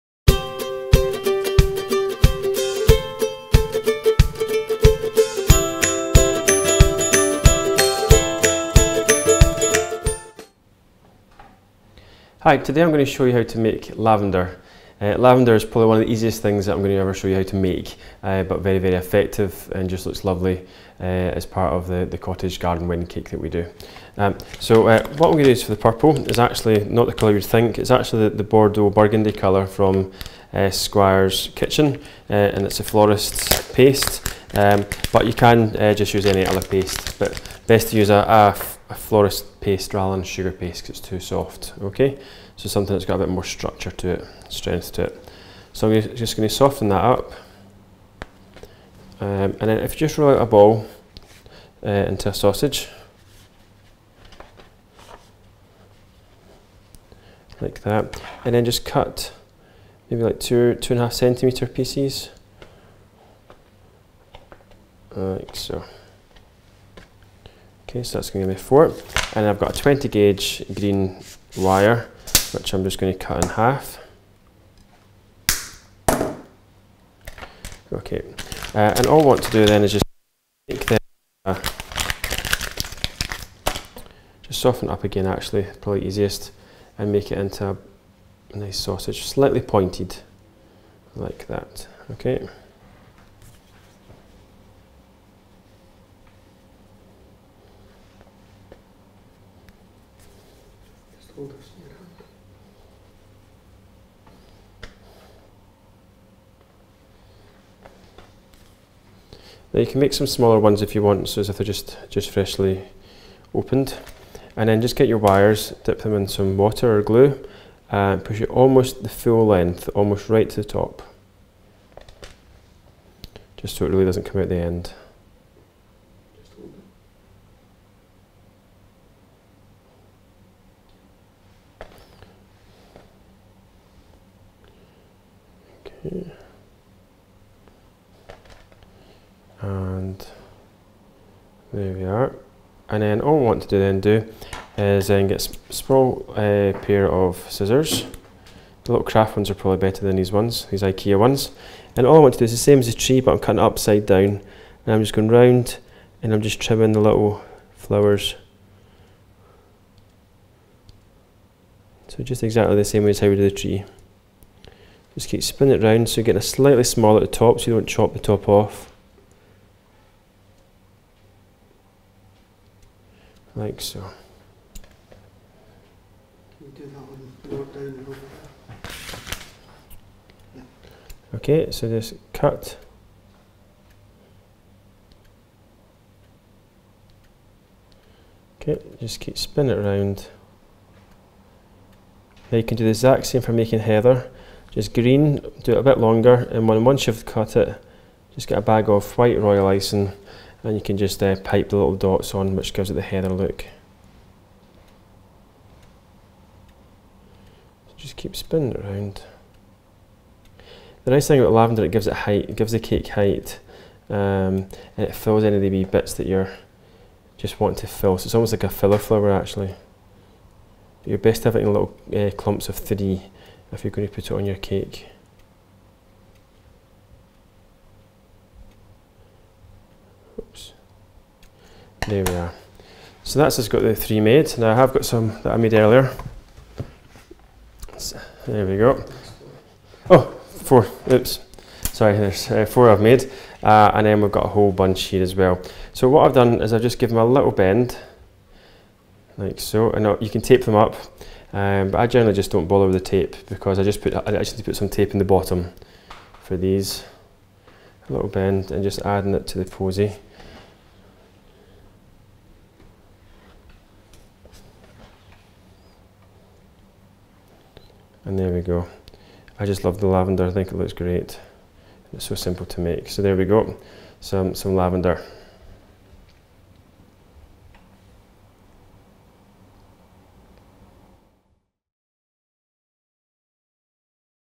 How to make edible lavender
Here Paul making sugar lavender to make some of the flowers which will make up the amazing woodland wedding cake.
There will be an array of flowers to give the overall look so watch this space for more coming soon.